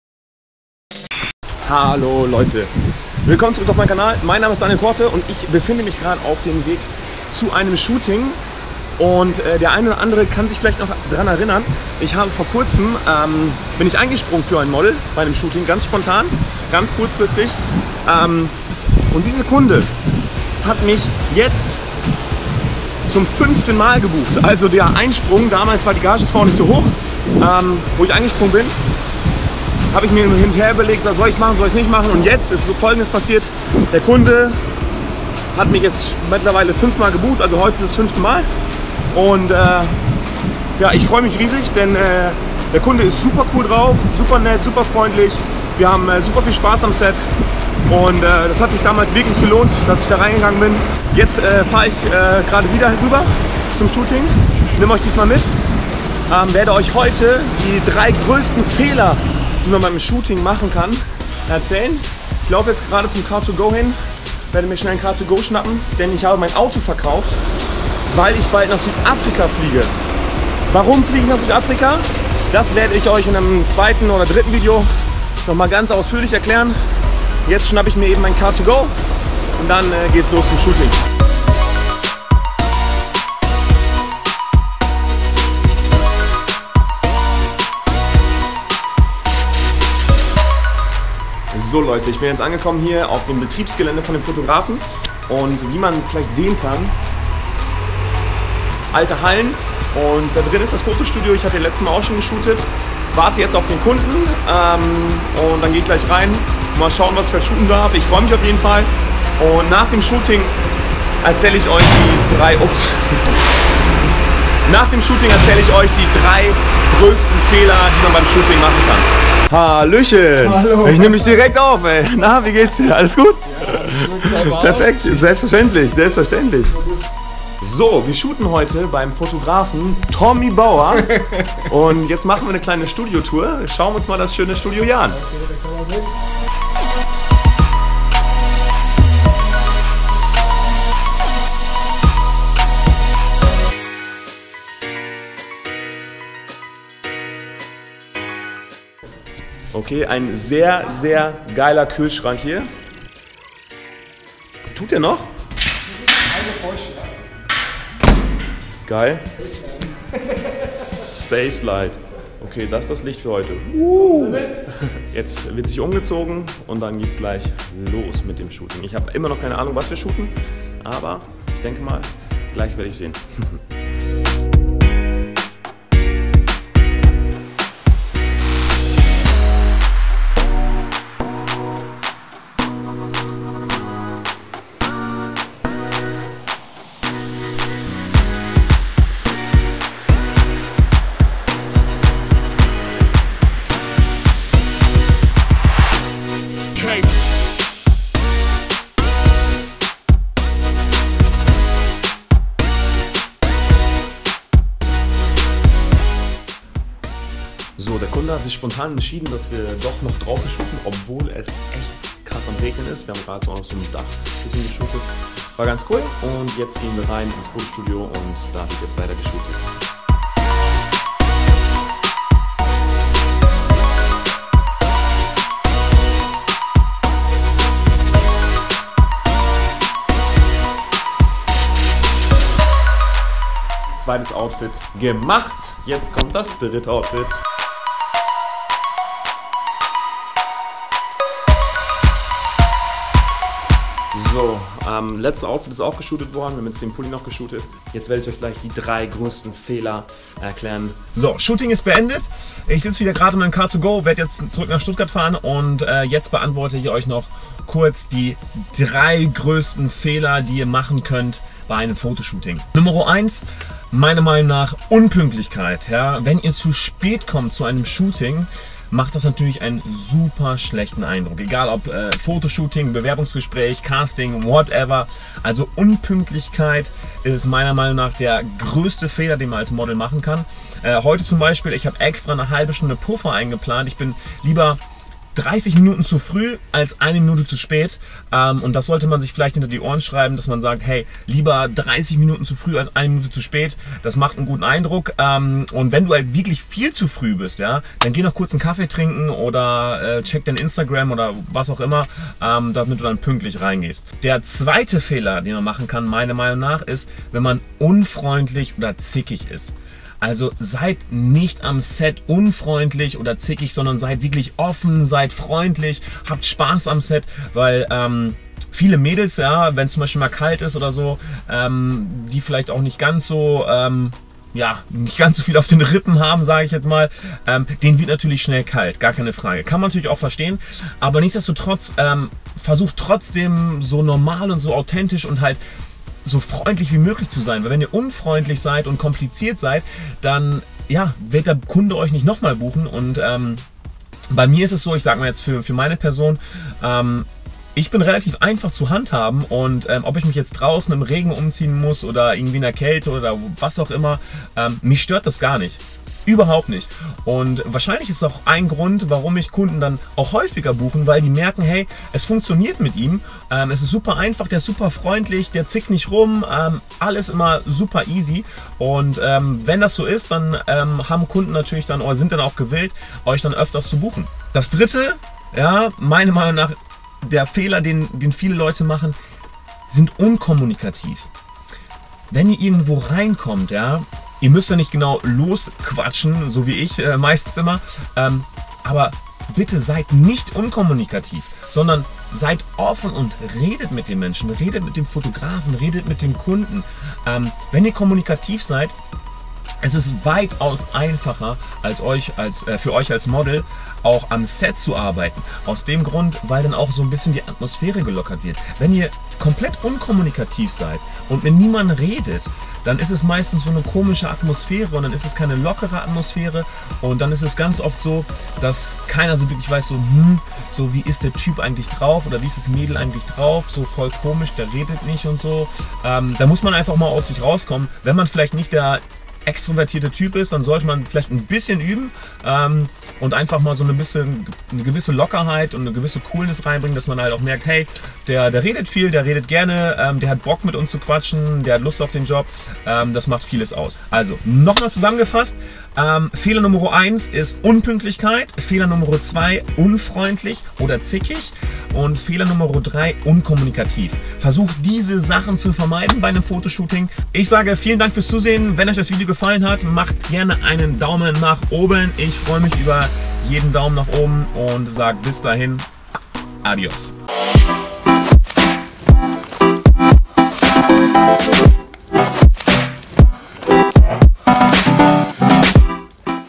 MODEL SHOOTING VLOG ● 3 FEHLER BEIM SHOOTING
Hey Leute!
Heute nehme ich euch mit zu einem Fotoshooting in der Nähe von Stuttgart! Als im Prinzip ein kurzer Vlog vom Shooting im Fotostudio beim Fotograf Tommy Bauer. Nachdem ich für ein Model eingesprungen bin, wurde ich nun öfter von dem Kunden als Männer Model gebucht. Das hat mich natürlich sehr gefreut, weil es ja auch immer ein Zeichen ist, dass die Arbeit und das Fotoshooting gut geklappt haben und der Kunde mit dem Model zufrieden war.
In dem Video erkläre ich euch noch die drei Fehler, die man als Model beim Fotoshooting unbedingt vermeiden sollte! Das ist extrem wichtig, gerade dann wenn man Model werden will und noch am Anfang der Modelkarriere steht. Also viel Spaß!
Peace